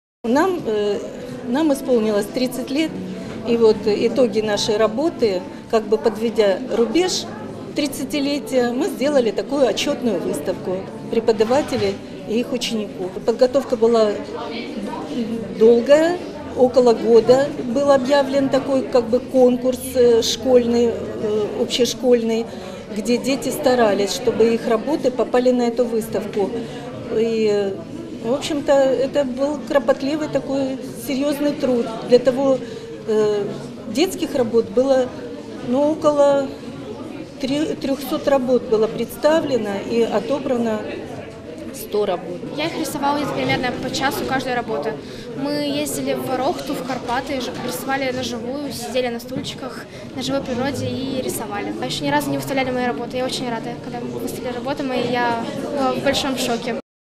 У Дніпрі відкрили виставку "Вчителі та учні"
У Дніпровському художньому музеї відкрилася виставка робіт

викладачів та учнів Дніпровської художньої школи №2 «Вчителі та учні».

Експозиція налічує понад сто  робіт у різних техніках декоративно-прикладного мистецтва, живопису та графіки. Підготовка до виставки тривала протягом року – ділиться директор художньої школи Ольга Бойченко. Взяти участь в такій експозиції надає можливість демонструвати свої роботи поруч з відомими майстрами фарби та пензля. Виставка «Вчителі та учні» працюватиме до 16-го лютого у виставкому залі

Дніпровського художнього музею, за адресою: вулиця  Шевченка, 21.